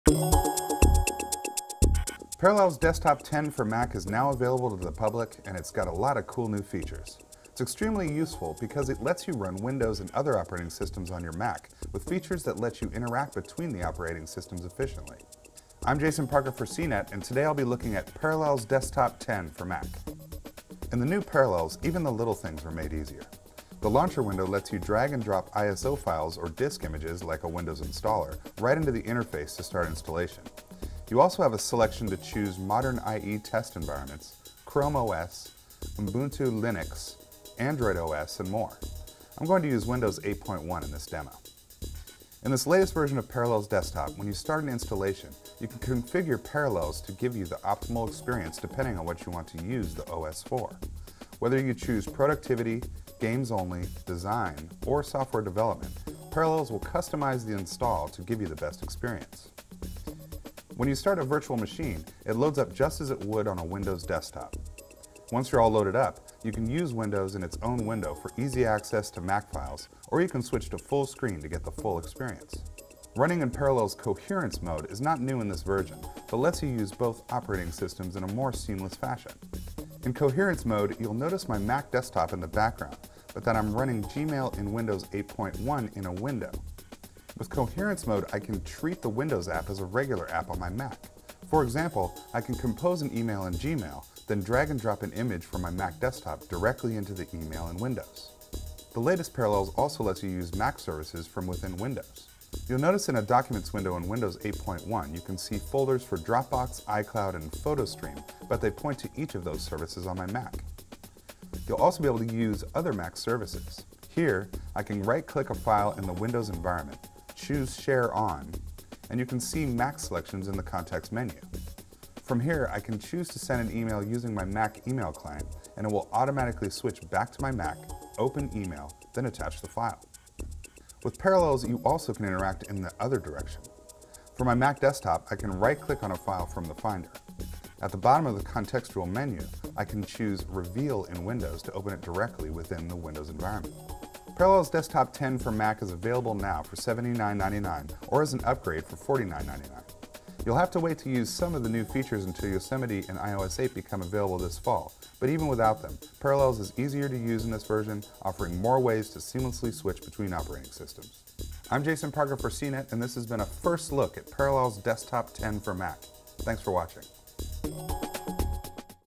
Easily run Windows and Mac OS simultaneously
Parallels Desktop 10 makes switching between operating systems a snap. CNET's Jason Parker takes a look.